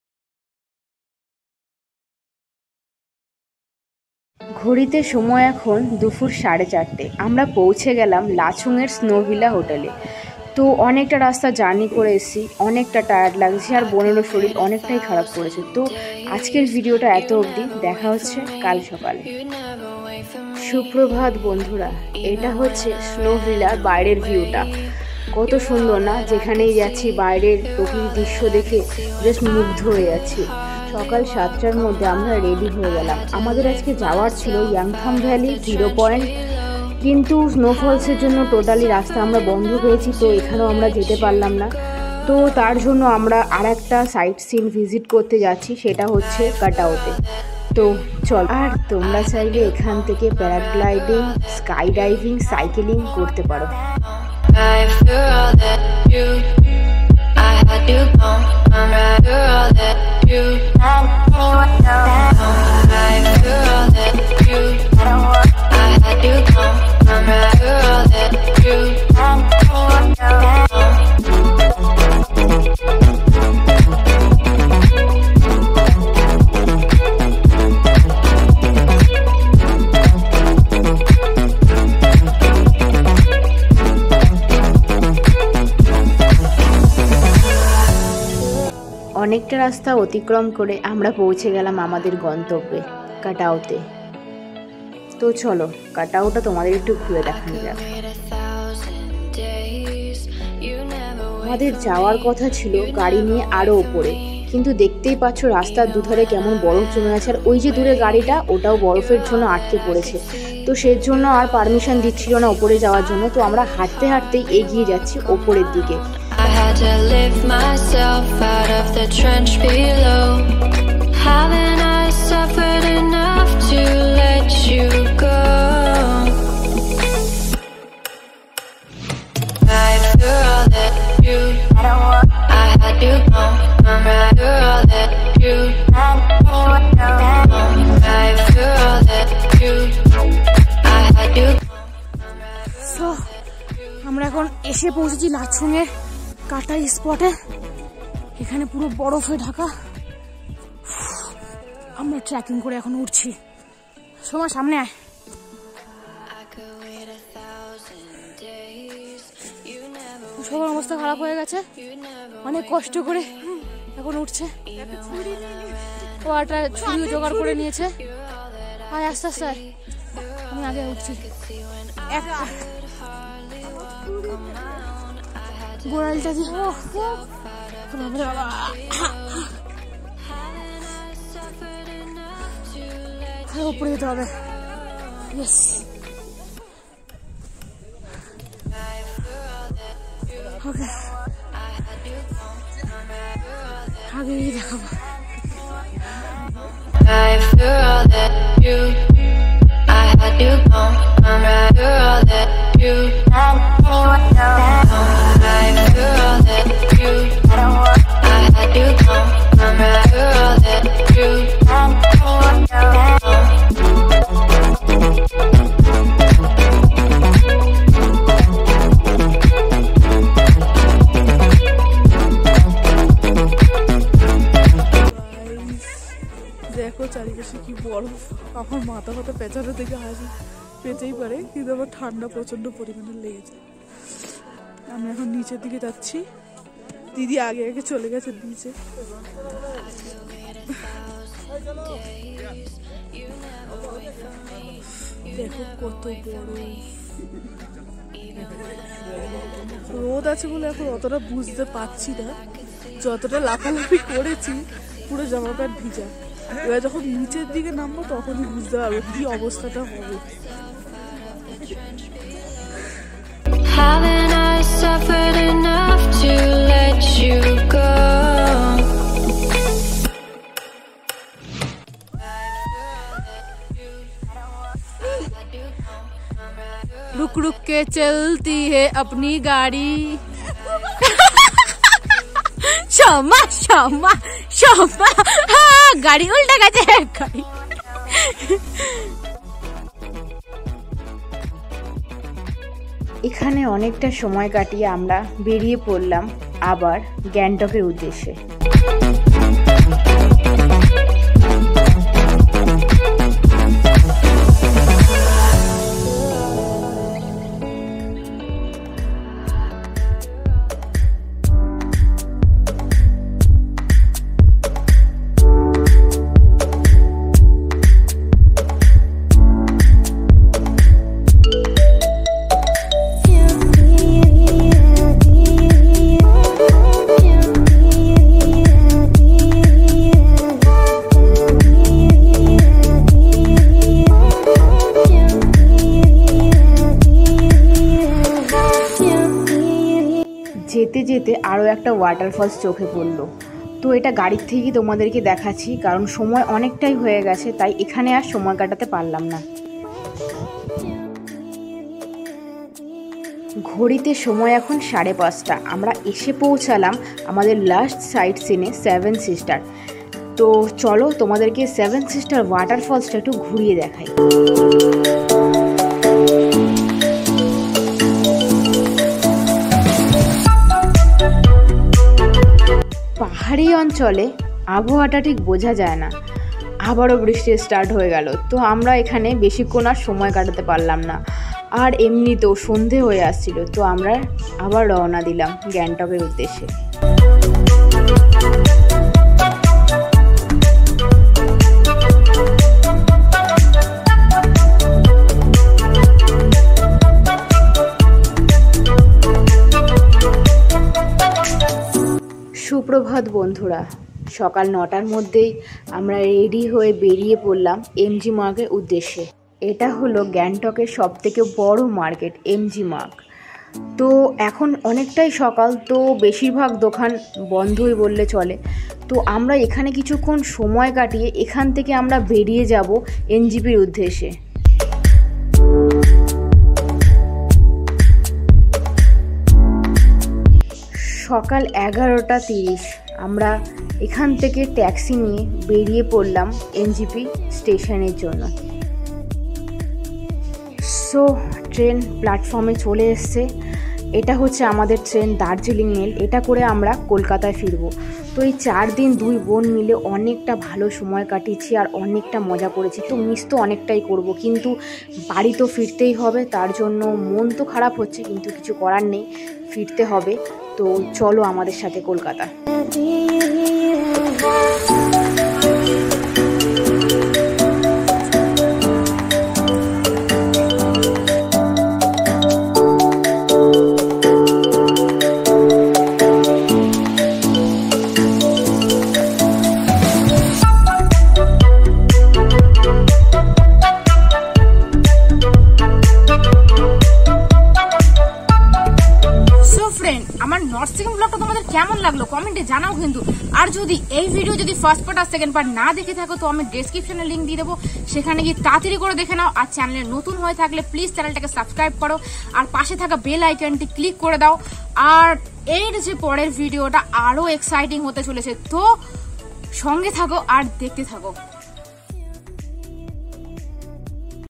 LACHUNG TO KOLKATA RETURN | Ep -3 | SIKKIM VLOG | Side Seen visit | CRAZY PINKY's VLOGS | 2022
Hello Kolkata...